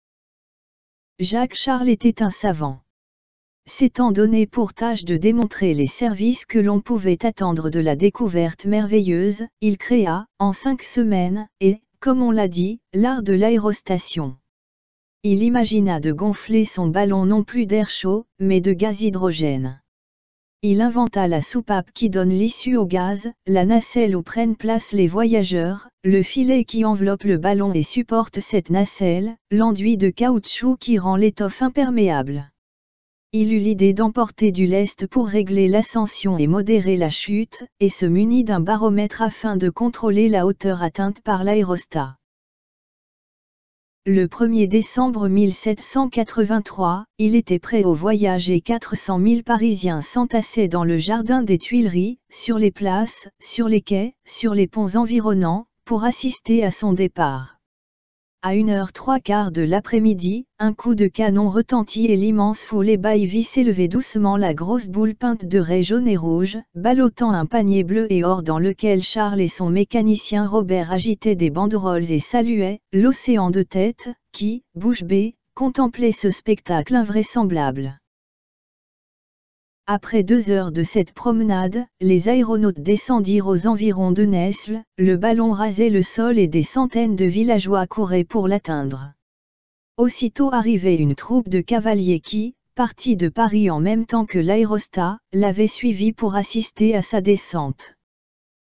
Jacques Charles 1783
vignette représentant la relation entre jacques charles et l'aéronautique